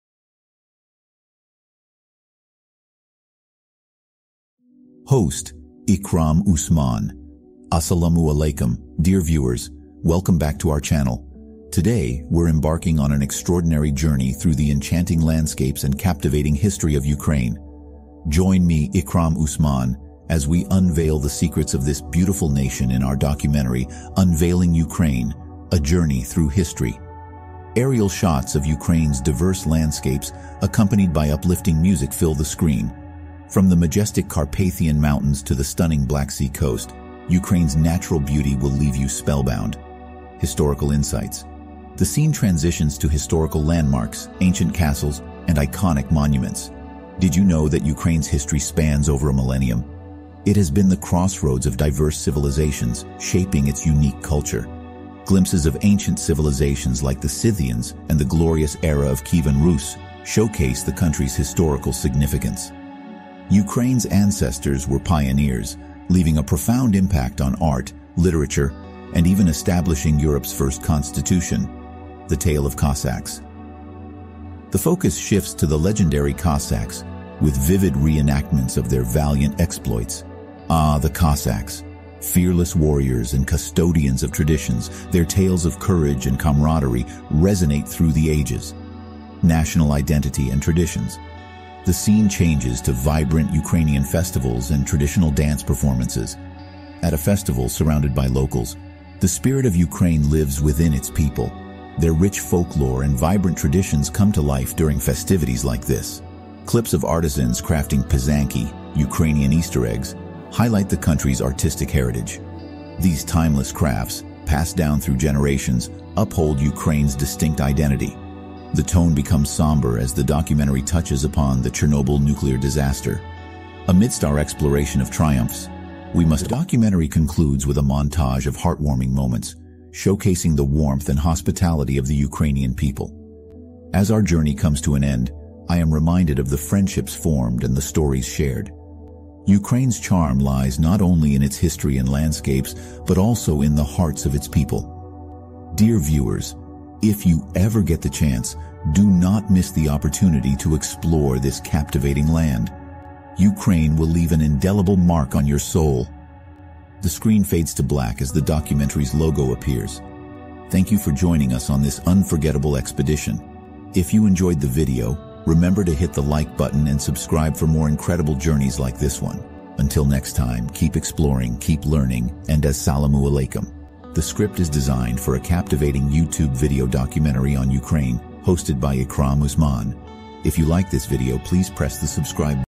"Discovering Ukraine: Unraveling Its Enigmatic Past and Captivating Present"
🚀 Get ready for an epic adventure as we take you on an extraordinary journey through the enigmatic land of Ukraine! 🎬 Our captivating documentary, "Unveiling Ukraine: A Journey Through History and Culture," will leave you spellbound with its breathtaking landscapes and rich heritage.

🏞️ Soar above the majestic Carpathian Mountains and be mesmerized by the azure shores of the Black Sea coast. 🌊 Experience the wonders of Ukraine's natural beauty like never before!

📜 Immerse yourself in the pages of history as we unravel the secrets of Ukraine's past. 🏰 From the legendary Cossacks to the glorious era of Kyivan Rus, witness the tales that shaped this nation's identity.

🎭 Dance to the beats of vibrant traditions and colorful festivals that celebrate Ukraine's rich cultural heritage. 🎉 Feel the warmth and hospitality of its people as we join in the jubilant celebrations.

☢️ Honor the resilience of Ukraine as we pay a solemn visit to the Chernobyl Exclusion Zone. 🕊️ This poignant segment serves as a reminder of the importance of safeguarding our planet and ensuring a safe future.

🍷 Indulge in the flavors of Ukraine's world-class wines and delectable cuisine. 🍇🍽️ From vineyards that produce exceptional wines to mouthwatering traditional dishes, your taste buds are in for a treat!

🎨 Witness the artistic soul of Ukraine through the masterful craftsmanship of local artisans. 🖌️ Admire the intricate designs of pysanky (Ukrainian Easter eggs) and be awed by contemporary artwork that captures the nation's creative spirit.

Join us on this thrilling and emotional ride through the heart of Ukraine - a nation that holds countless stories and endless wonders. 🌍 Don't miss "Unveiling Ukraine: A Journey Through History and Culture" - a visual masterpiece that will ignite your wanderlust and curiosity! 📽️🌟